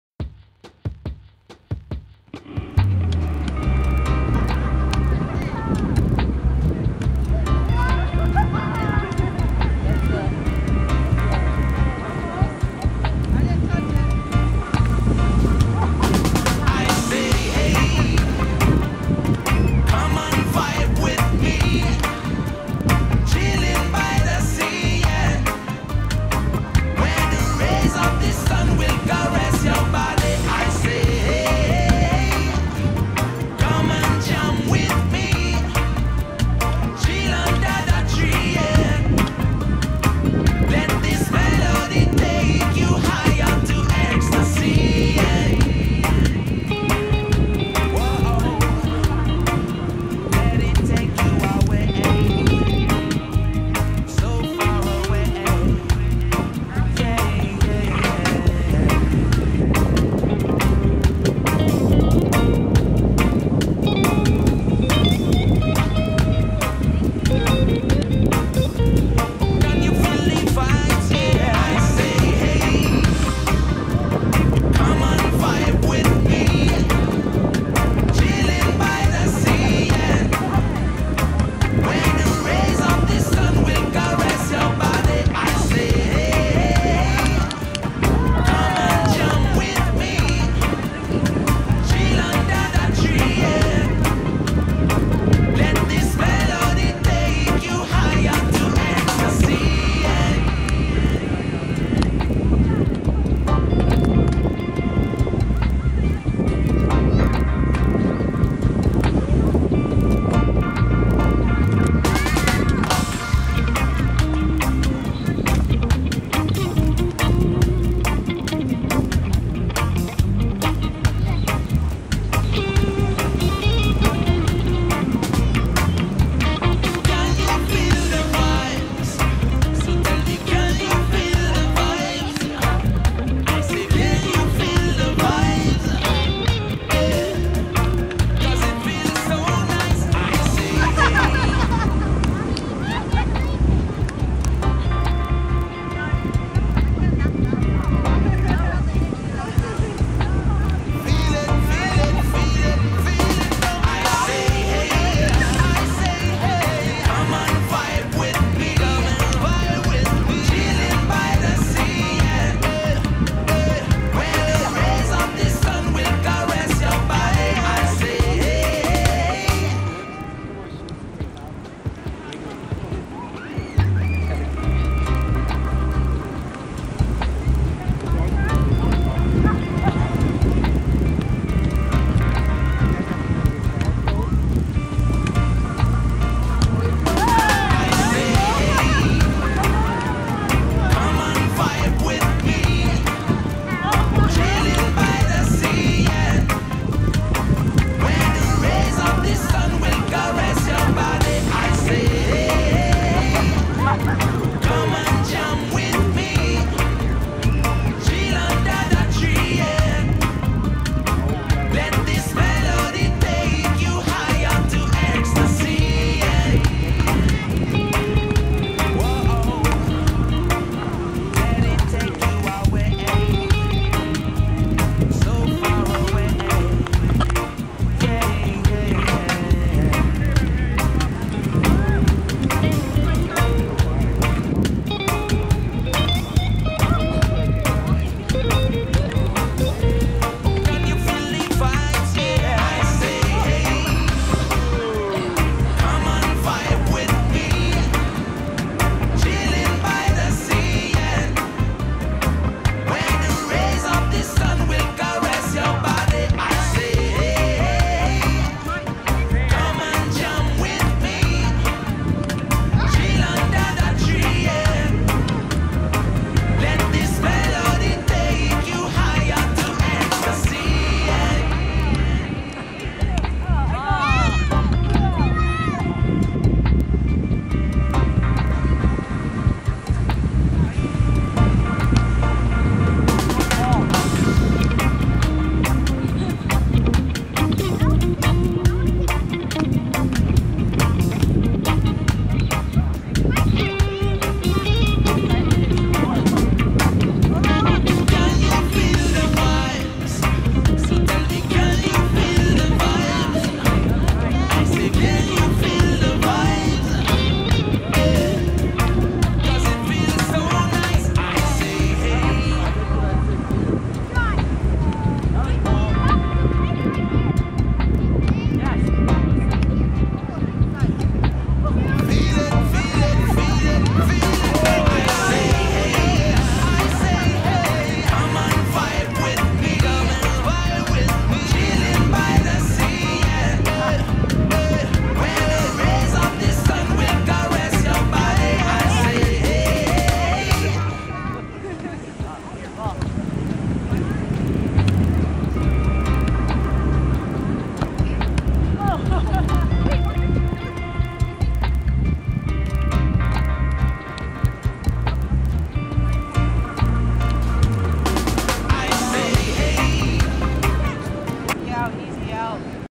Girls Beach Volleyball - July 29, 2017
I play vball maybe once a year.  So out of shape and I am paying for it right now, my body is sore from head to toe but it was so much fun!  My lovely friends kicking my butt.  I had to "time out"so that I could catch my breath.  LOL.